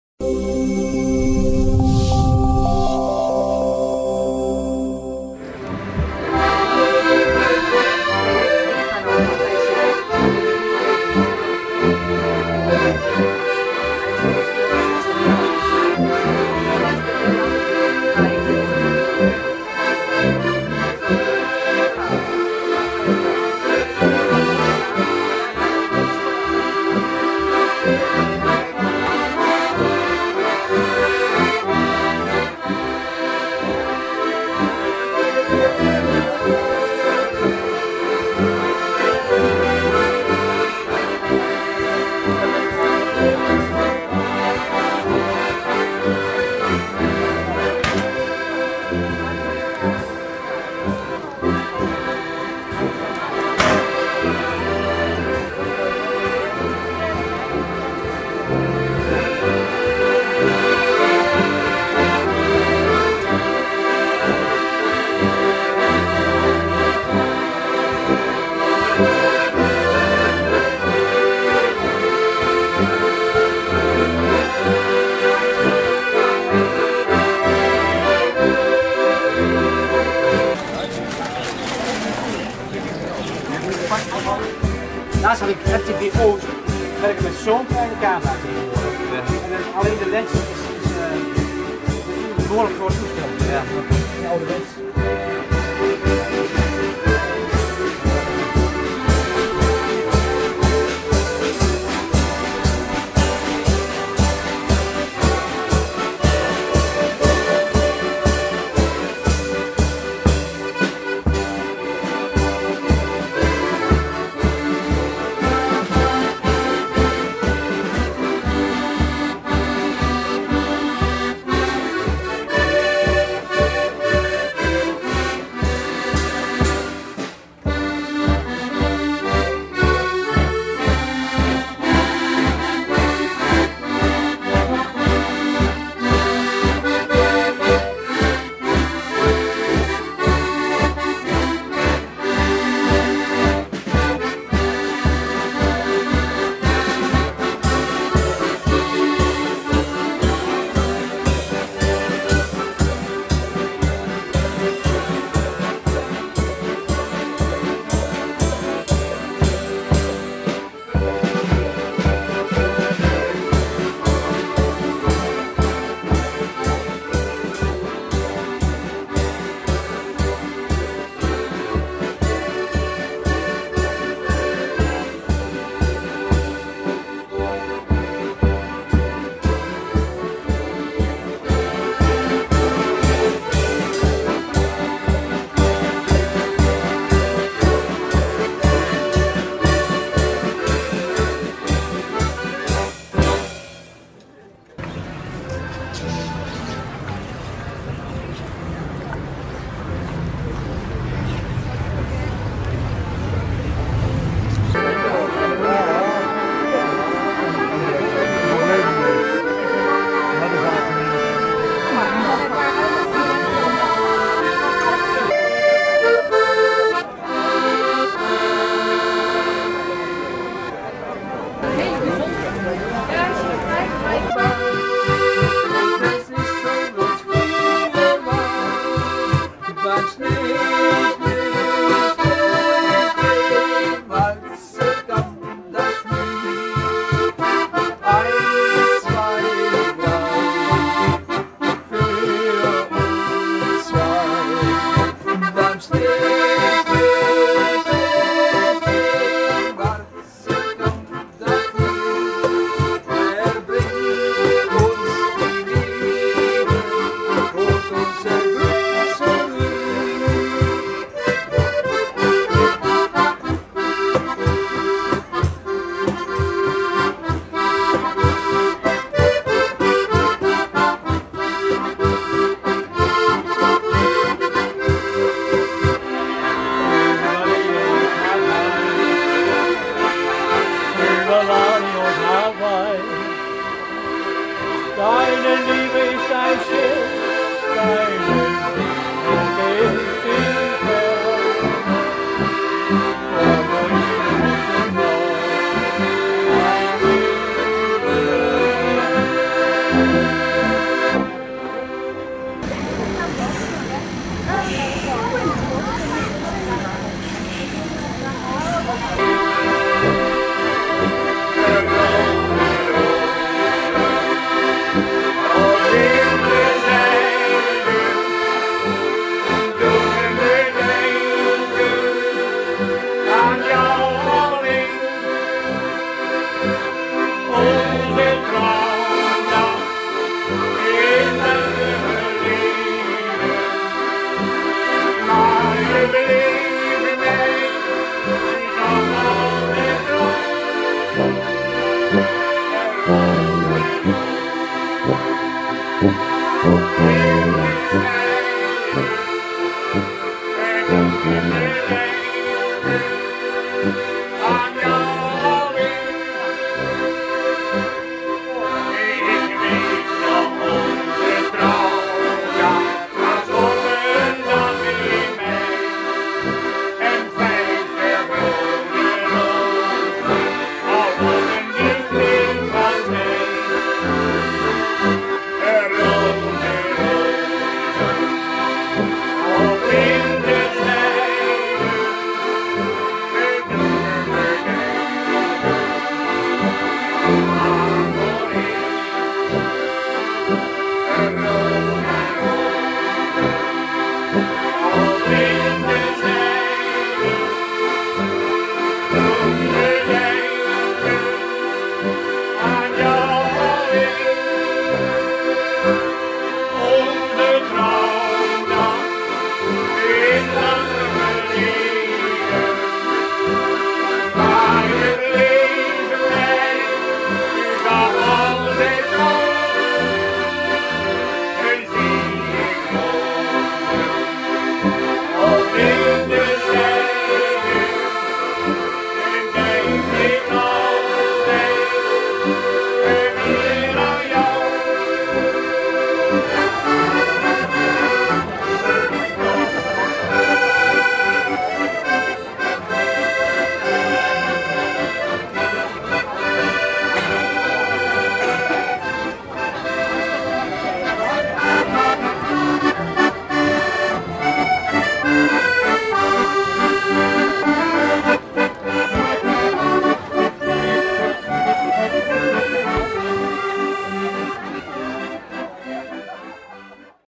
Hardenberg: Harmonicadag - gezelligheid troef bij De Zwiese
Gezelligheid was zaterdag troef bij Landhoeve De Zwiese waar de jaarlijkse harmonicadag werd gehouden. Op het terrein waren diverse podia ingericht waar harmonicaspelers als groep of individueel muziek konden maken. Er werd volop gebruik van gemaakt. Het geheel werd omlijst met een gezellige markt.
* Te zien op televisie via wijZijn.tv en via Internet op alle apparaten op of via Instagram.